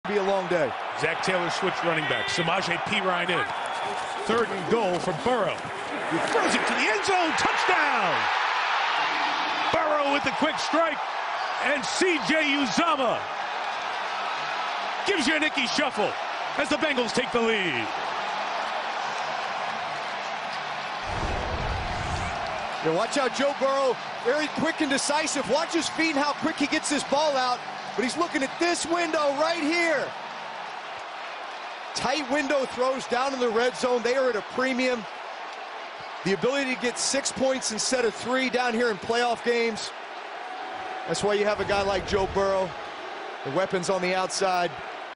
Joe Burrow TD Pass to C J Uzomah (First Touchdown of Playoffs)
Joe Burrow TD Pass to C J Uzomah. Bengals get on the scoreboard first